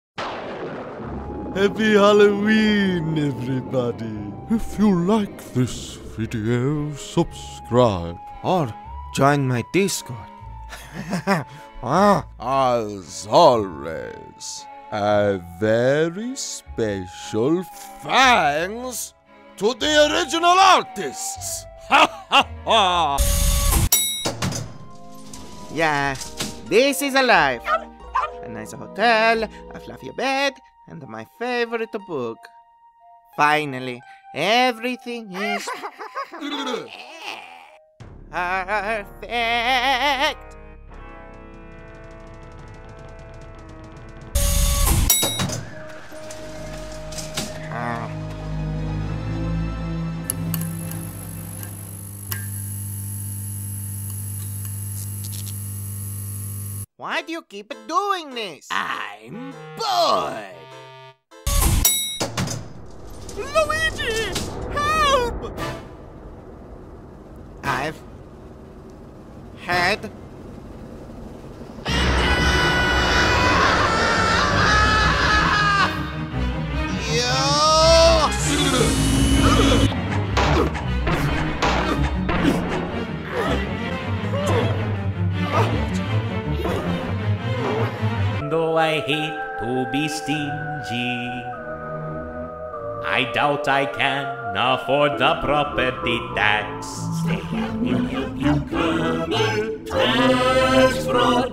Luigi's Mansion 3 Comic Dub Compilation!! - Mario Brothers' Luigi Comic Dub
Video edited by me and TomatoMagica
Links to comics below the cut